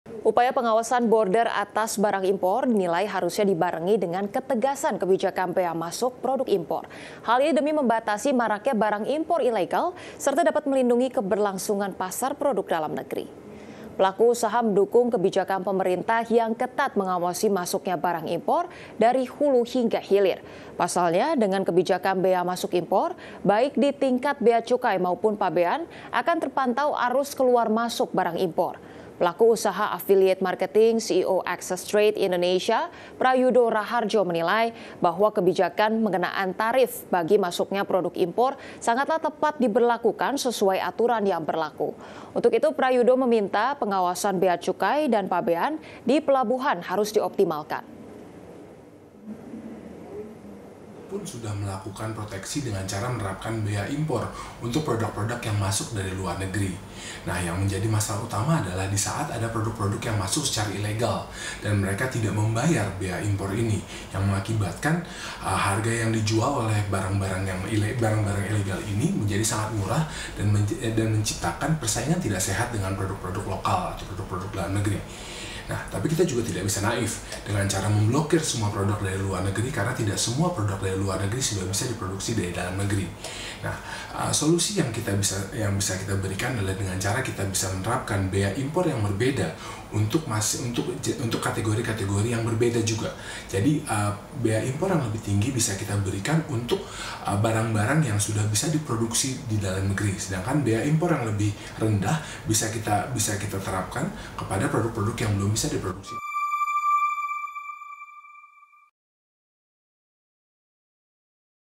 Bea Masuk Impor Harus Dioptimalkan | IDX CHANNEL
Upaya pengawasan border atas barang impor, dinilai harusnya dibarengi dengan ketegasan kebijakan pea masuk produk impor. Hal ini demi membatasi maraknya barang impor ilegal, serta dapat melindungi keberlangsungan pasar produk dalam negeri.